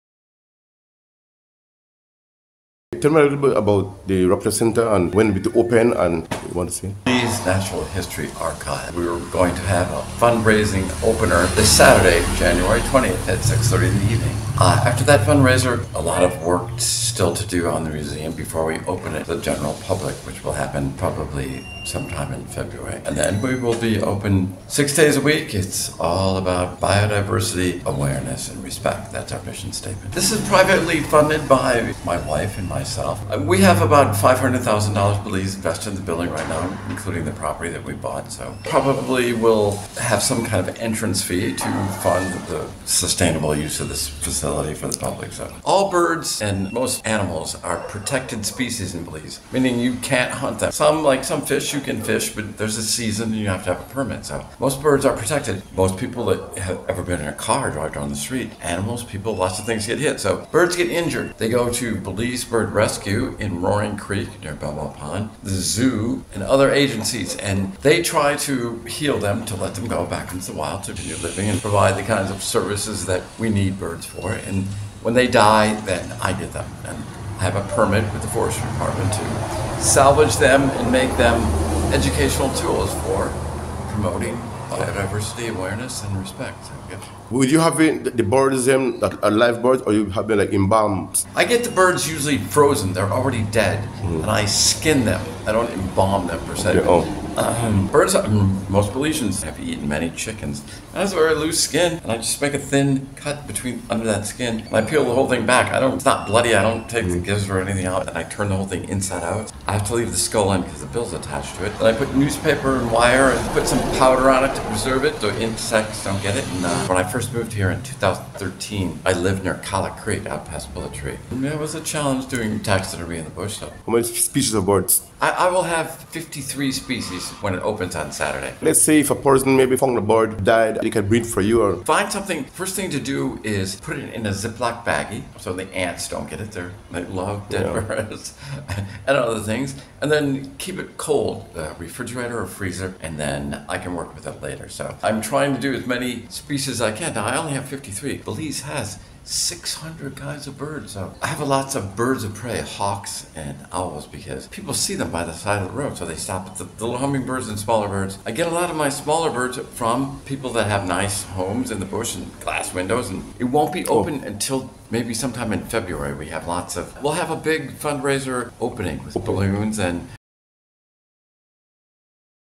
DR STEPHEN ZITZER
Fundraising opening ceremony for the rapture building in Santa Elena town